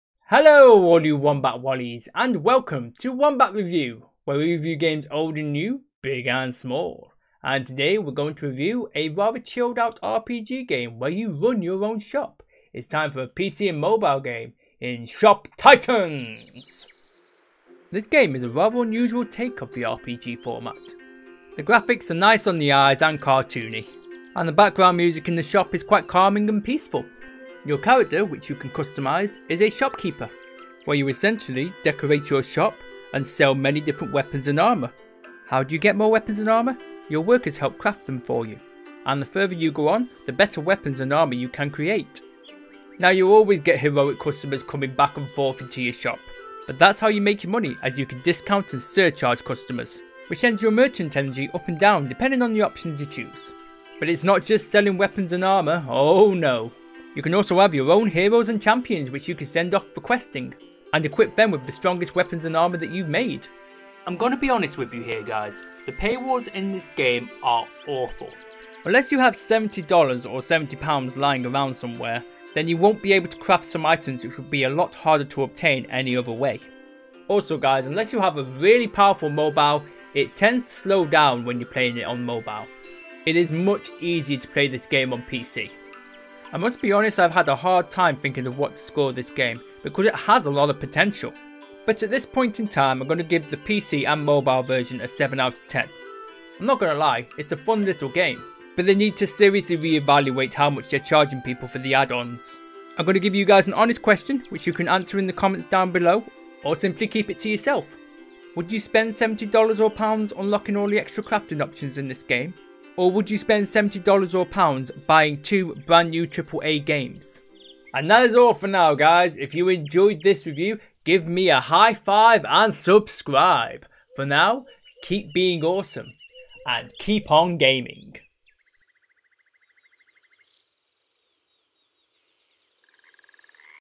Wombat Review: Shop Titans
A good game that can get better! The ball's in your court Kabam!